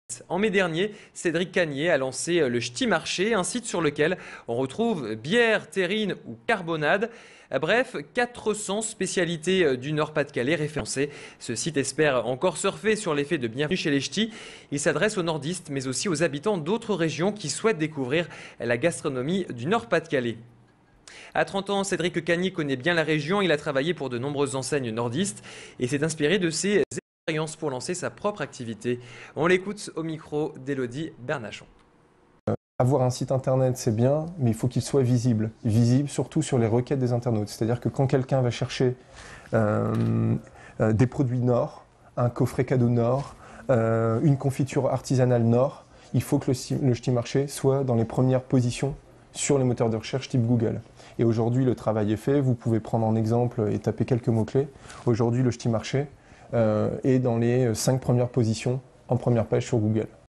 Ch'ti Marché : Le terroir du Nord s'étend sur le Web !
Vous voulez conserver ce reportage ?
 Focus sur le site Internet Ch'ti Marché, spécialisé dans la vente de produits régionaux du Nord Pas de Calais.
. (DROITS RESERVES . Pour toute exploitation sur TV, web, mobile, aller